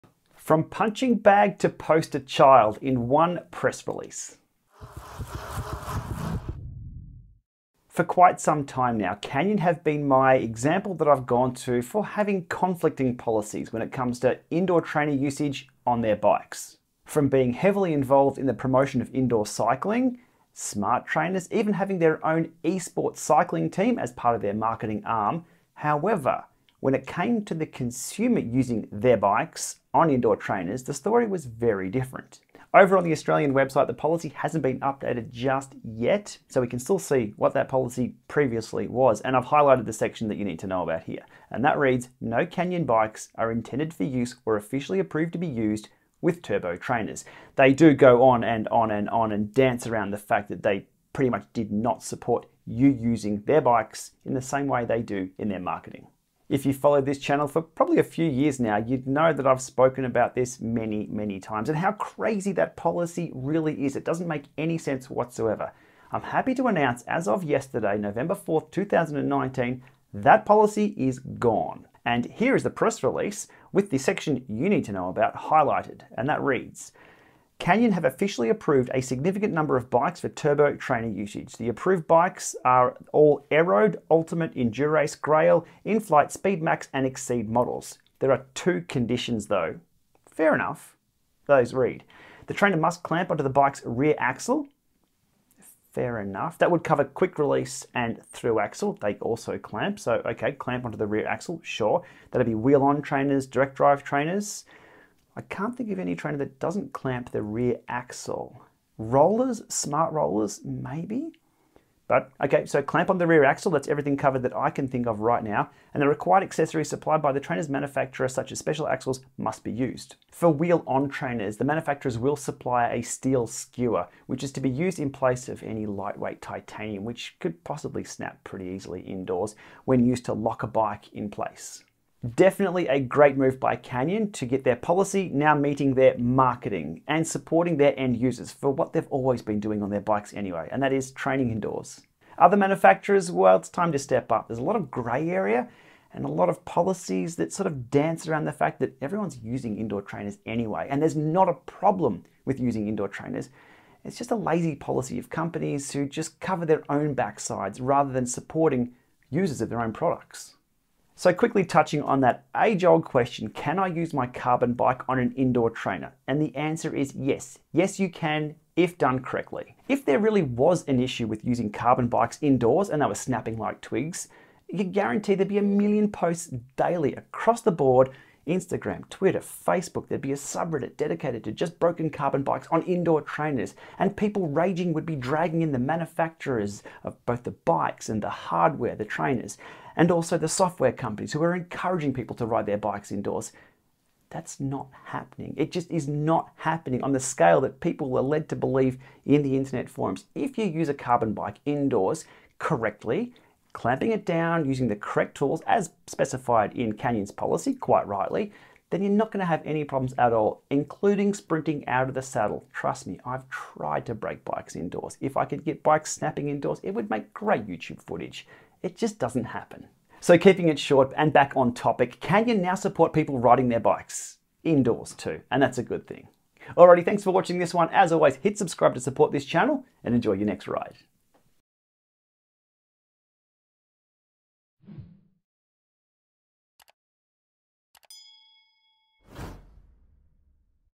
Canyon Officially Approves Bikes for Turbo Trainer Usage
Very welcome news from a Canyon press release on Nov 4th 2019: Canyon have officially approved a significant number of bikes for turbo trainer usage. The approved bikes are all Aeroad, Ultimate, Endurace, Grail, Inflite, Speedmax, and Exceed models. There are only two conditions: the trainer must clamp on to the bike's rear axle, and required accessories supplied by the trainer's manufacturer (such as special axles) must be used.